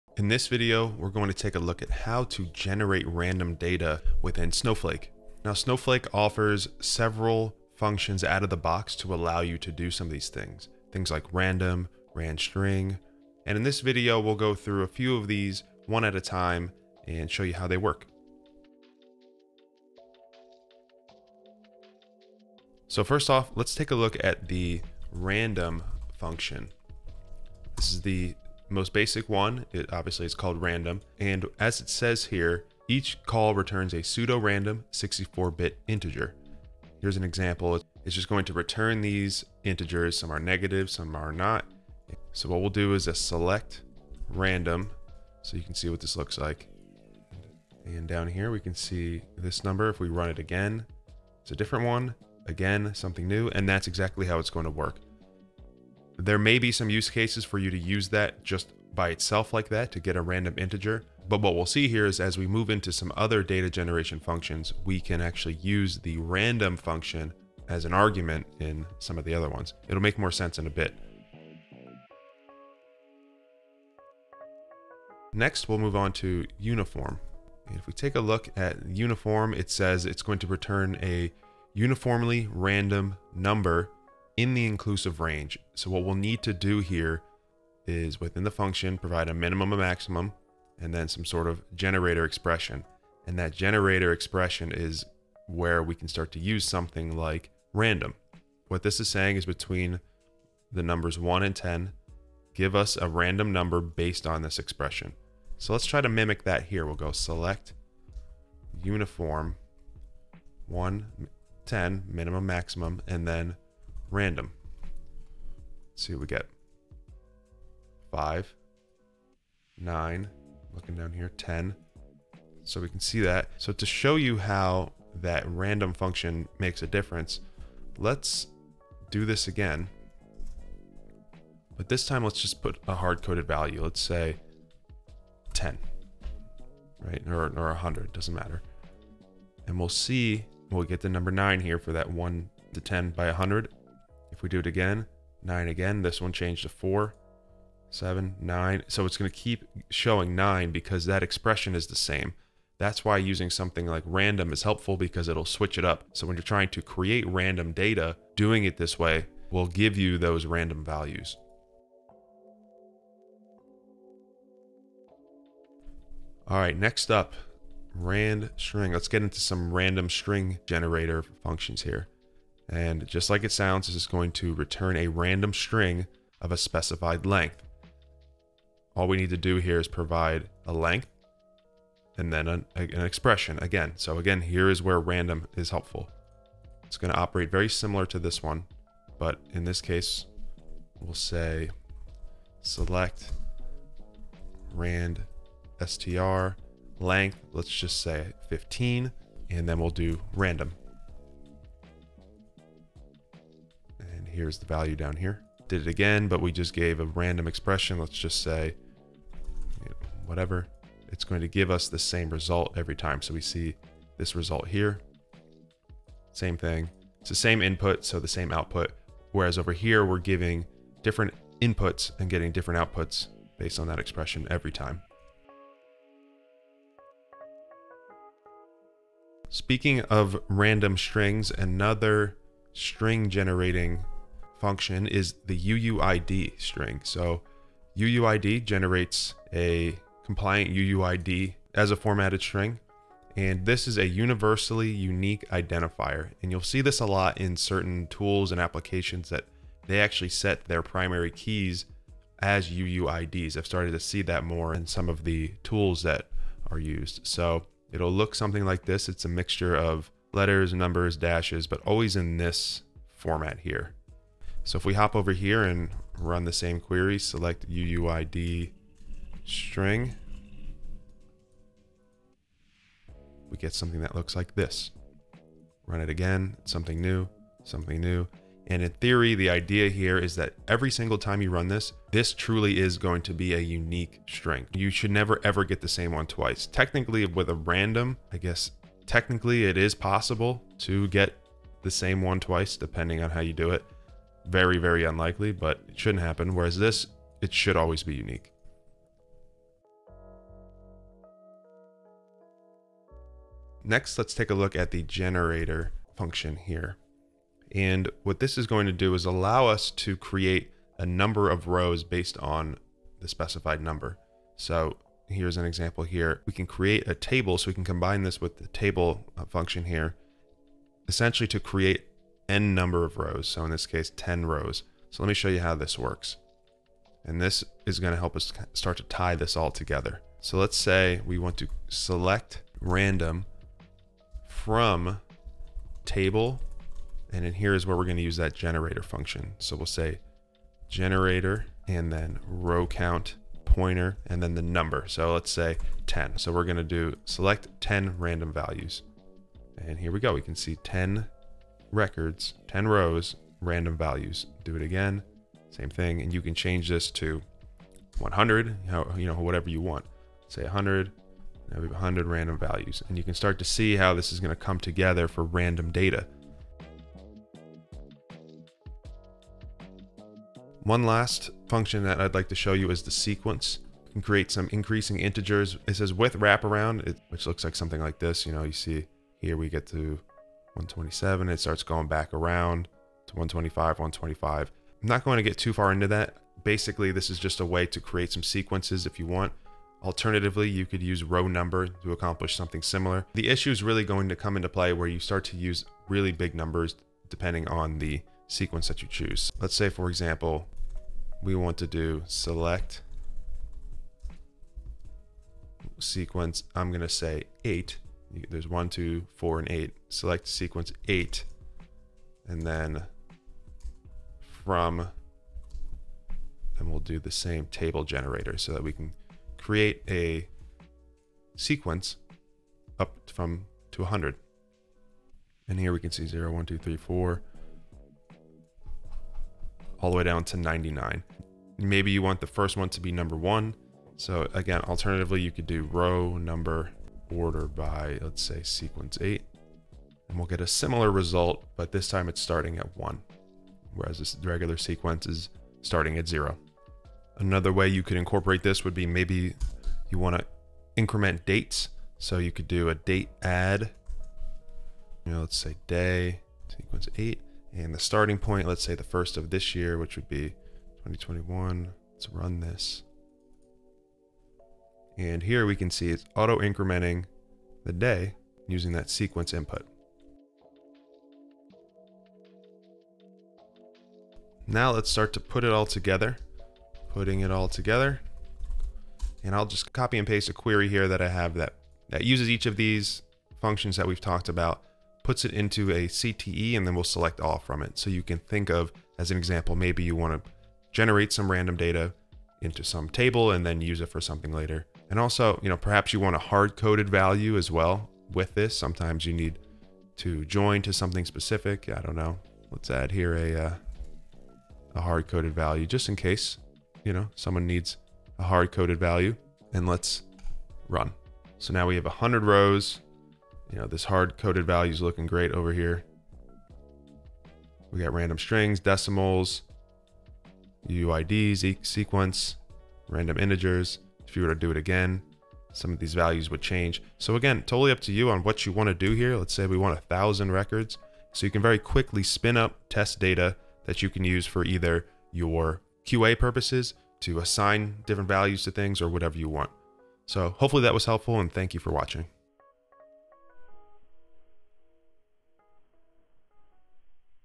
Data Generation w/ Snowflake
Whether for QA or for other purposes, at times you may want to quickly create random data comprised of various data types. Snowflake offers some built-in functions to make this process very easy and scalable.

In this video, we will walk through some of the most common data generation functions so that you can become more comfortable using them as needed.

Data Generation functions allow you to generate data. Snowflake supports two types of data generation functions:

- Random, which can be useful for testing purposes.
These functions produce a random value each time. Each value is independent of the other values generated by other calls to the function. The underlying algorithm produces pseudo-random values, and thus the values are not truly random or independent, but without knowing the algorithm, the values are essentially unpredictable, usually evenly distributed (if the sample size is large), and pseudo-independent of each other.

- Controlled distribution, which can be useful for providing unique ID numbers for records that do not already have unique identifiers.
These functions produce values that are not independent. For example, the NORMAL function returns values that have an approximately “normal” (bell-shaped) distribution based on a specified mean and standard deviation. Thus, each new value generated is at least indirectly influenced by previously generated values as the function tries to maintain the specified distribution. As another example, the SEQ family of functions return a sequence of values.

Timestamps:
00:00 - Intro
00:22 - Random Function
01:12 - Uniform Function
02:42 - RandStr Function
03:45 - UUID_String Function
05:00 - Generator Function
06:07 - SEQ Function
07:51 - Putting It All Together

Title & Tags:
How to Generate Data (random) in Snowflake | Data Generation Functions | Snowflake Tutorial